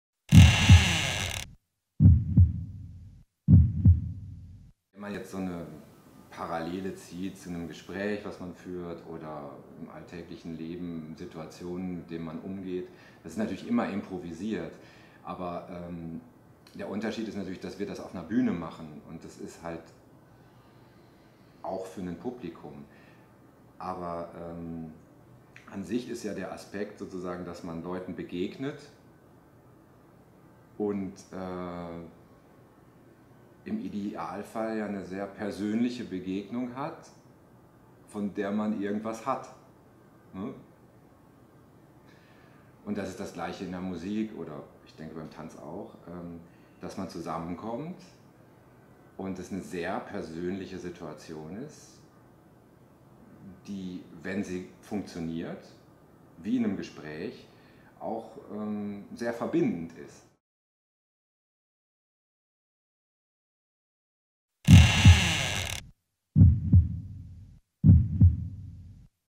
about improvisation: Matthias Muche
Short interview on free improvisation in music & dance.
This film is part of the BLIND DATE project realized by Sebastian Weber and Sebastian Göschel at the LOFFT Leipzig 2016.

Camera & Edit: Sophie Stephan